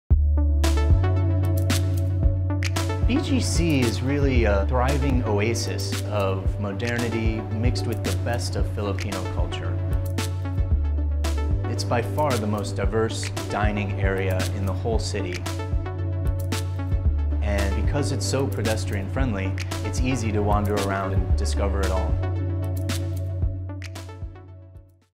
Shangri-La at the Fort, Manila - BCG
BGC is a thriving oasis of modernity mixed with the best of Filipino culture: warm, friendly, spirited and heartfelt. Here, you’re at the center of business and commerce, and some of Asia’s best night life. It’s also peaceful, and the landscaped greenery make it a wonderful place to live, work and play.